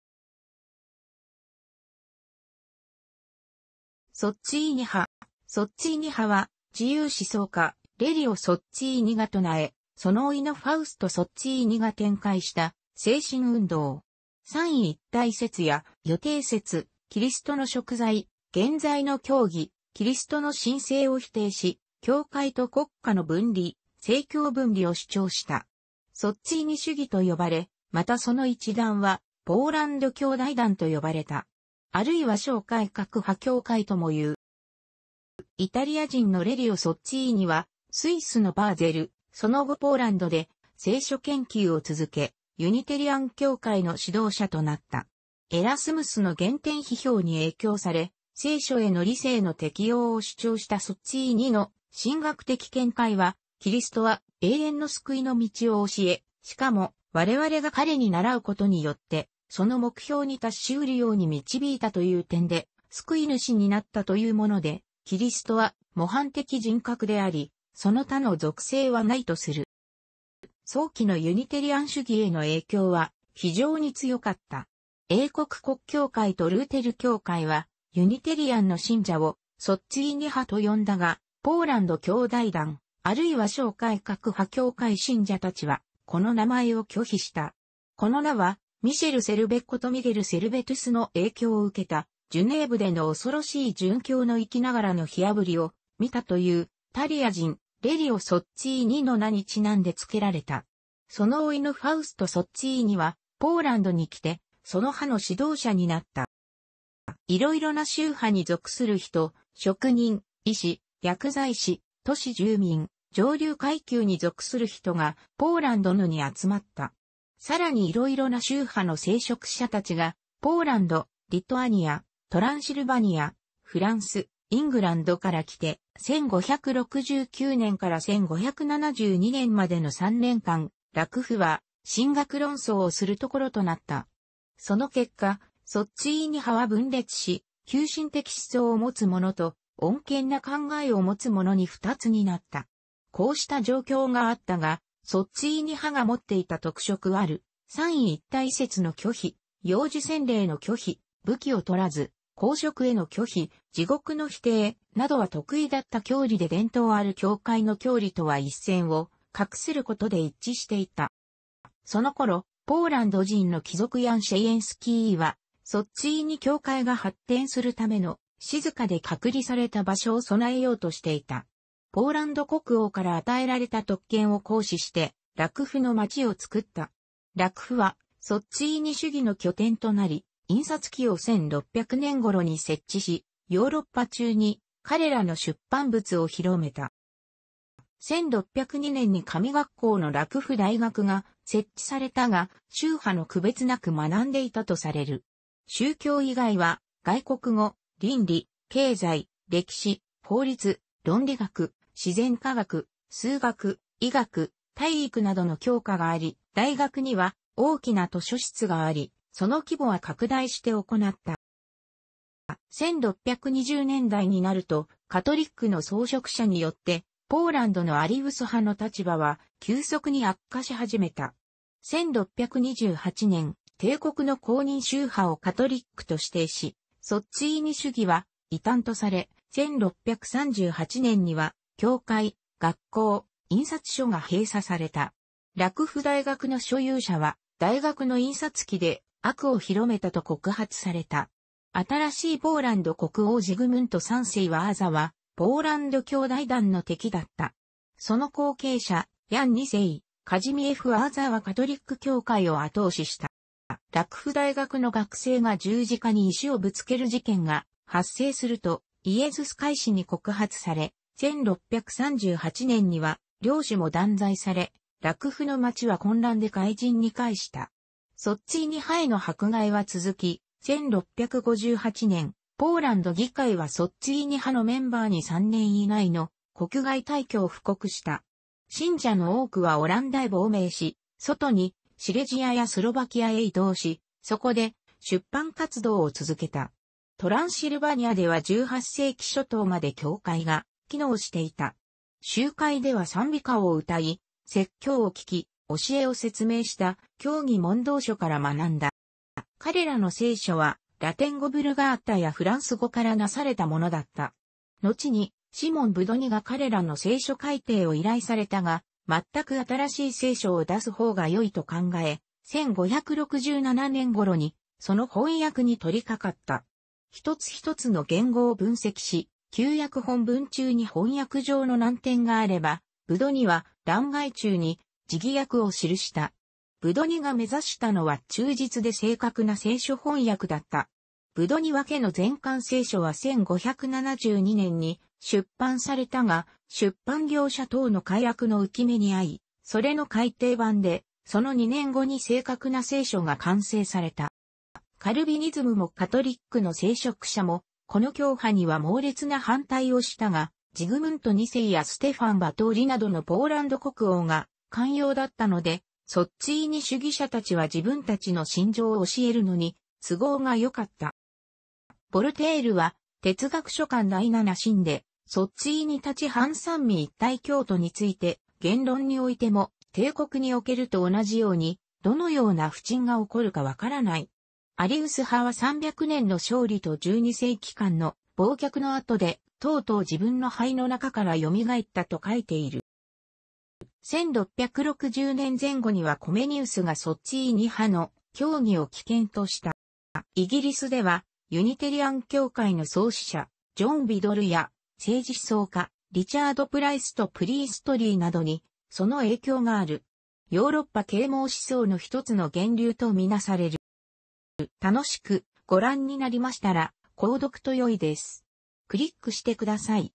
ソッツィーニ派
ソッツィーニ派は、自由思想家レリオ・ソッツィーニが唱え、その甥のファウスト・ソッツィーニが展開した精神運動。三位一体説や予定説、キリストの贖罪、原罪の教義、キリストの神性を否定し、教会と国家の分離（政教分離）を主張した。ソッツィーニ主義と呼ばれ、またその一団はポーランド兄弟団と呼ばれた。或いは小改革派教会ともいう。

イタリア人のレリオ・ソッツィーニはスイスのバーゼル、その後ポーランドで聖書研究を続け、ユニテリアン教会の指導者となった。エラスムスの原典批評に影響され、聖書への理性の適用を主張したソッツィーニの神学的見解は、「キリストは永遠の救いの道を教え、しかもわれわれが彼にならうことによって、その目標に達しうるように導いたという点で救い主になった」というもので、キリストは模範的人格であり、その他の属性はないとする。

早期のユニテリアン主義への影響は非常に強かった。英国国教会とルーテル教会はユニテリアンの信者を「ソッツィーニ派」と呼んだが、ポーランド兄弟団、或いは小改革派教会信者たちは、この名前を拒否した。この名はミシェル・セルヴェことミゲル・セルベトゥスの影響を受けた、ジュネーヴでの恐ろしい殉教の生きながらの火あぶりを見たというイタリア人レリオ・ソッツィーニの名にちなんで付けられた。その甥のファウスト・ソッツィーニはポーランドに来て、その派の指導者になった。

色々な宗派に属する人、職人、医師、薬剤師、都市住民、上流階級に属する人がポーランドのに集まった。さらに色々な宗派の聖職者たちがポーランド、リトアニア、トランシルヴァニア、フランス、イングランドから来て、1569年から1572年までの3年間、ラクフは神学論争をする所となった。その結果、ソッツィーニ派は分裂し、急進的思想を持つ者と、穏健な考えを持つ者に二つになった。こうした状況があったが、ソッツィーニ派が持っていた特色ある、三位一体説の拒否、幼児洗礼の拒否、武器を取らず、公職への拒否、地獄の否定、などは特異だった教理で伝統ある教会の教理とは一線を画することで一致していた。

その頃、ポーランド人の貴族ヤン・シェイエンスキーはソッツィーニ教会が発展するための「静かで隔離された場所」を備えようとしていた。ポーランド国王から与えられた特権を行使して、ラクフの町をつくった。ラクフはソッツィーニ主義の拠点となり、印刷機を1600年頃に設置し、ヨーロッパ中に彼らの出版物を広めた。

1602年に神学校のラクフ大学が設置されたが、宗派の区別無く学んでいたとされる。宗教以外は外国語、倫理、経済、歴史、法律、論理学、自然科学、数学、医学、体育などの教科があり、大学には大きな図書室があり、その規模は拡大して行った。

1620年代になるとカトリックの僧職者によって、ポーランドのアリウス派の立場は急速に悪化し始めた。1628年、帝国の公認宗派をカトリックと指定し、ソッツィーニ主義は異端とされ、1638年には教会、学校、印刷所が閉鎖された。ラクフ大学の所有者は大学の印刷機で『悪を広めた』と告発された。新しいポーランド国王ジグムント3世ワーザはポーランド兄弟団の敵だった。その後継者ヤン2世カジミエフ・ワーザはカトリック教会を後押しした。

ラクフ大学の学生が十字架に石をぶつける事件が発生すると、イエズス会士に告発され、1638年には領主も断罪され、ラクフの街は混乱で灰燼に帰した。ソッツィーニ派への迫害は続き、1658年、ポーランド議会はソッツィーニ派のメンバーに3年以内の国外退去を布告した。信者の多くはオランダへ亡命し、外にシレジアやスロバキアへ移動し、そこで出版活動を続けた。トランシルバニアでは18世紀初頭まで教会が機能して居た。集会では賛美歌を歌い、説教を聴き、教えを説明した教義問答書から学んだ。

彼らの聖書はラテン語ヴルガータやフランス語から為されたものだった。のちに、シモン・ブドニが彼らの聖書改訂を依頼されたが、全く新しい聖書を出す方が善いと考え、1567年頃にその翻訳に取り掛かった。一つ一つの原語を分析し、旧約本文中に翻訳上の難点が在れば、ブドニは欄外注に字義訳を記した。ブドニが目指したのは忠実で正確な聖書翻訳だった。ブドニ訳の全巻聖書は1572年に出版されたが、出版業者等の改悪の憂き目に合い、それの改訂版でその2年後に正確な聖書が完成された。

カルヴィニズムもカトリックの聖職者も、この教派には猛烈な反対をしたが、ジグムント二世やステファ...